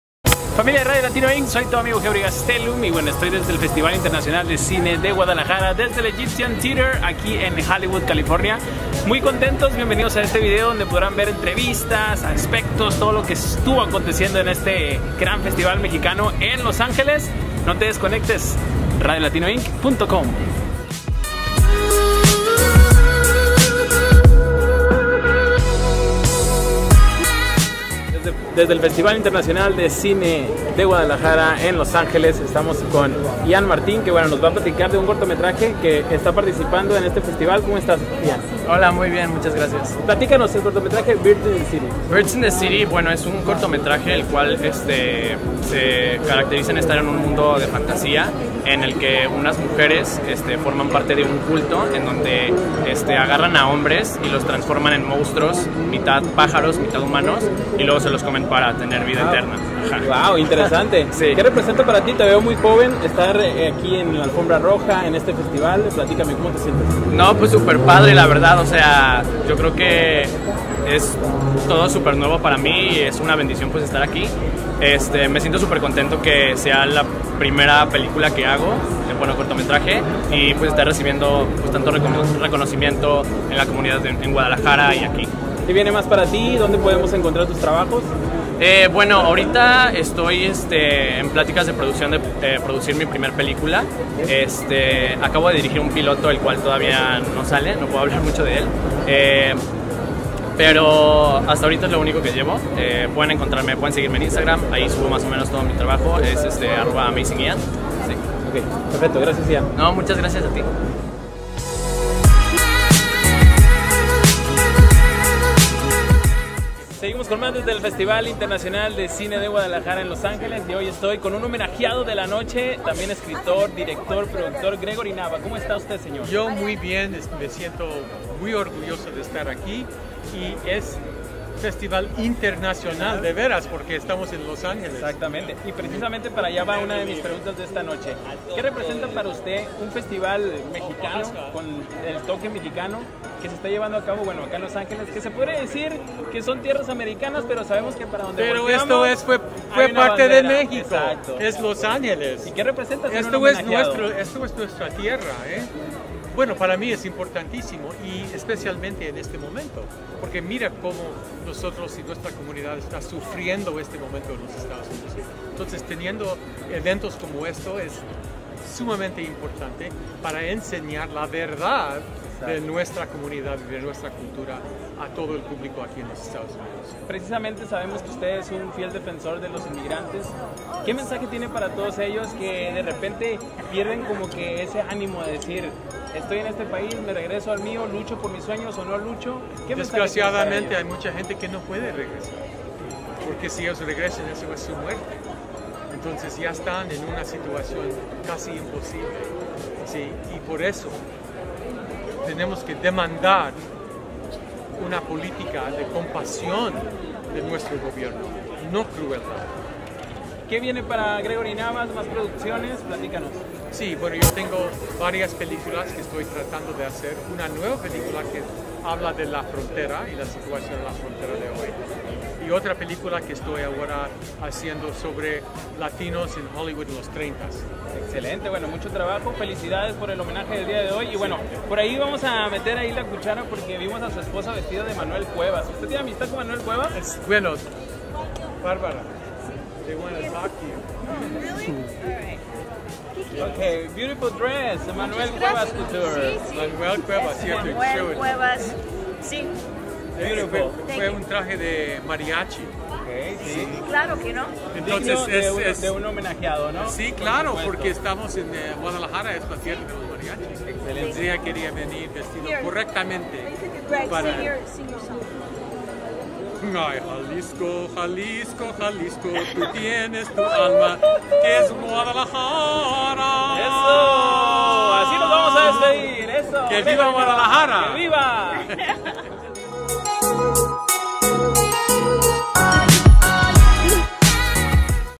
FICGLA ‘19 | Festival de Cine en Los Ángeles | Radio Latino INC
Asistimos a la alfombra roja del Festival de Cine de Guadalajara en Los Ángeles donde tuvimos oportunidad de platicar con Gregory Nava, director y productor homenajeado de la noche en GregoryNava

Radiolatinoinc.com
Instagram: @RadioLatinoINC   FB: Radio Latino INC

Selena (1997) Full MOVIE'